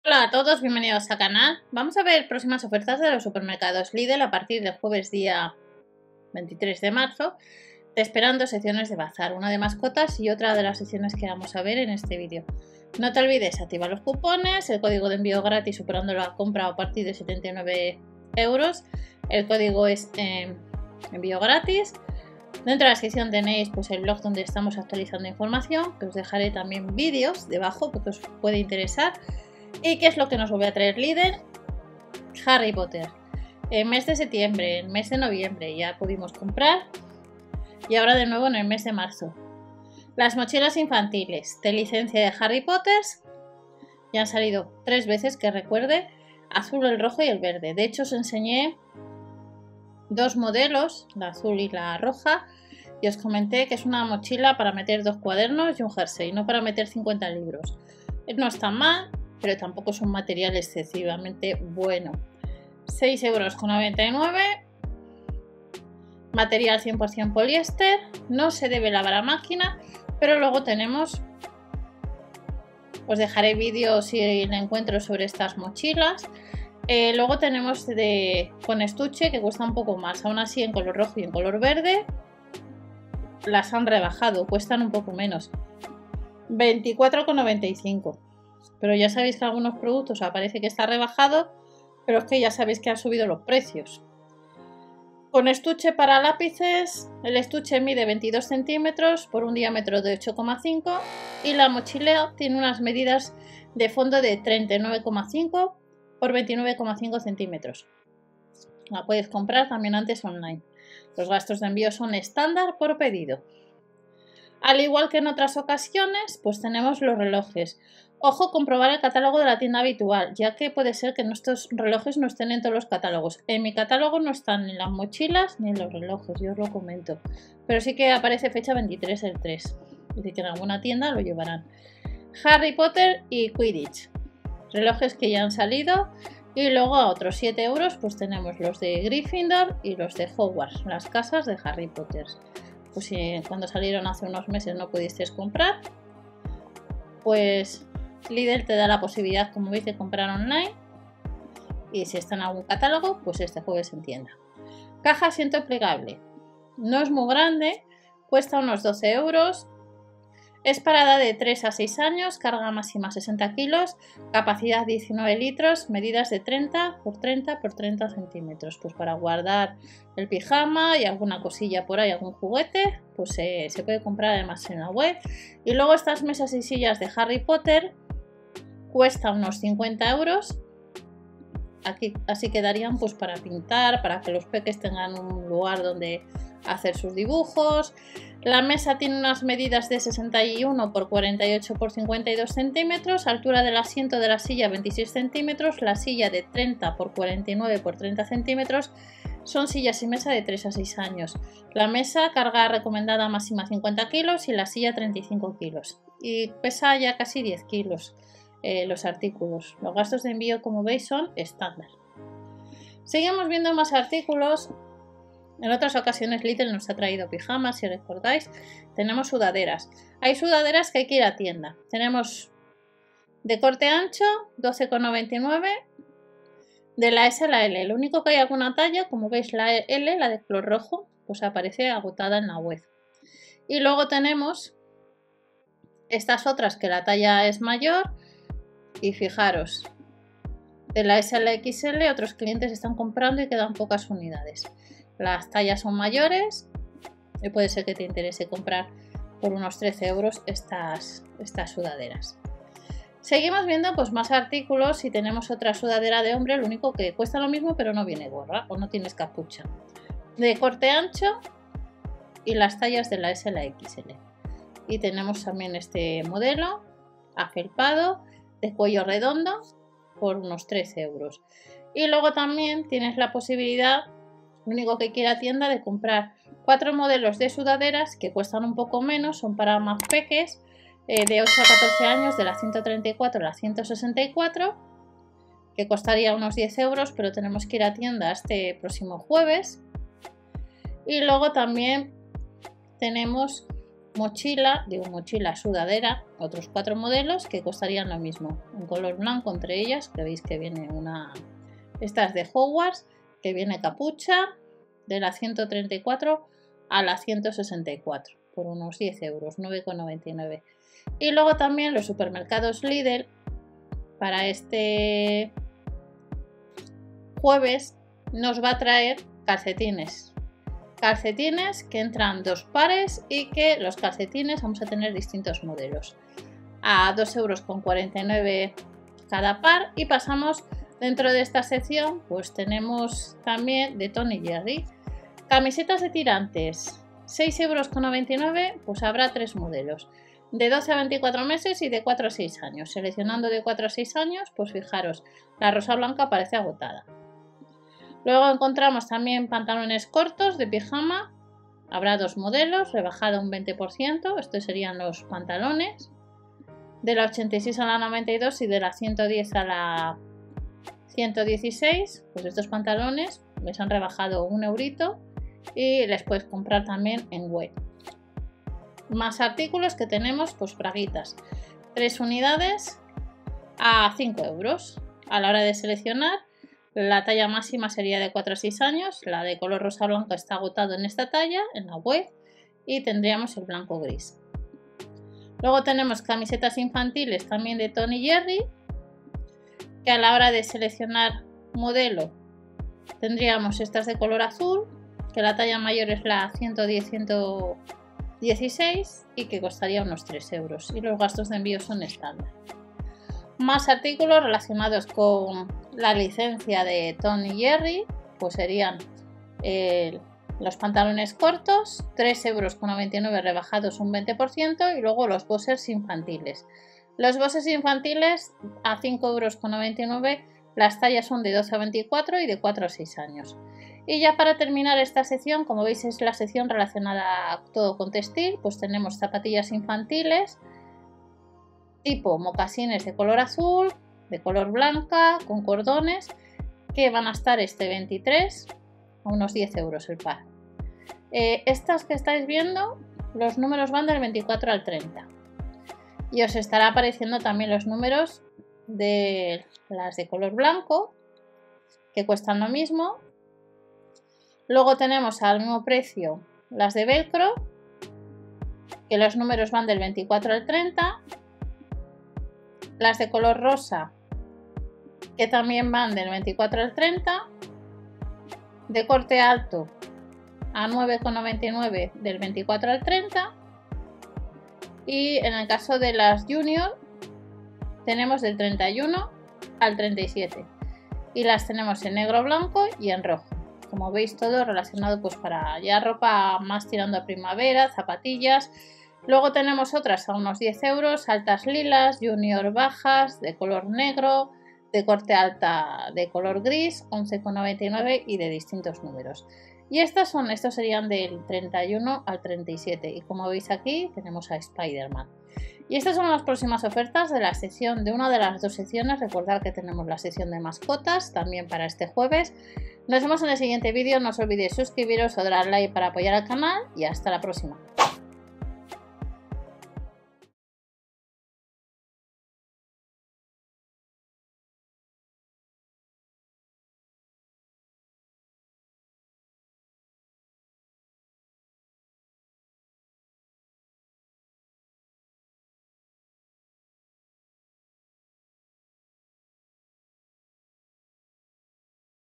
LIDL EL PRÓXIMO JUEVES 23/03
Ofertas desde el 23 de Marzo en los supermercados LIDL

También puedes ayudar al canal con el botón de GRACIAS.

OTRA INFORMACIÓN

✅ CANAL @OfertasPromocionesySorteos
✅ CANAL   @msguelytv

TIME LINE DEL VÍDEO

00:00 - 00:39Información
00:40 - 05:34 Jueves 23/03
05:35 - 07:02 2ª Sección
07:03 - 09:03 3ª sección y despedida

📝  www. msguely.info